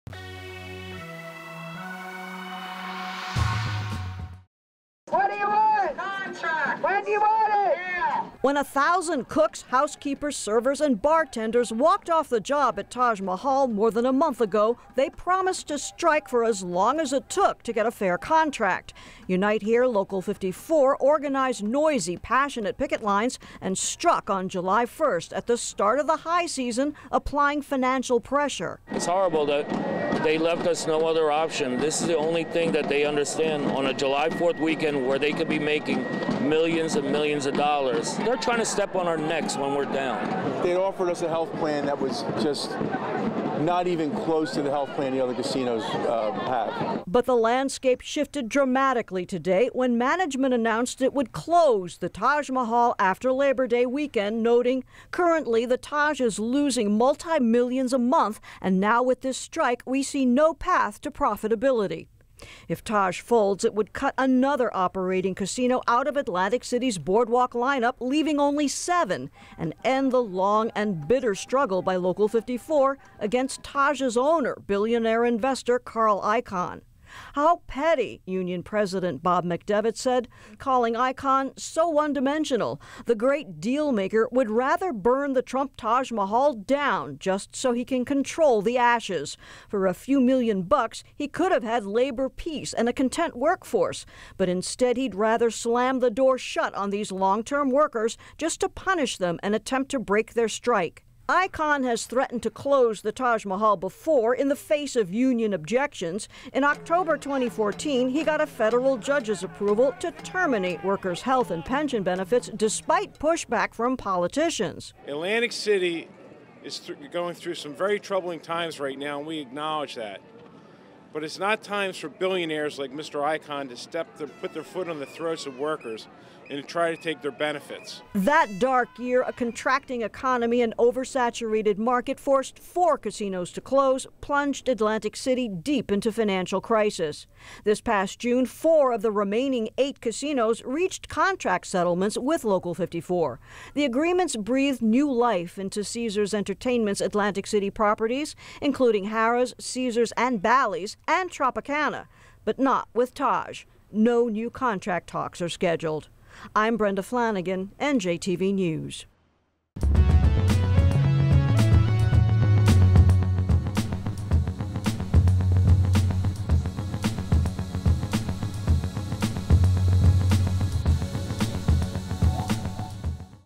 Taj Mahal Management Plans to Close Casino After Labor Day
Management announced it would close the Taj Mahal after Labor Day weekend, noting it is losing multi-millions a month and with the strike there is no path to profitability.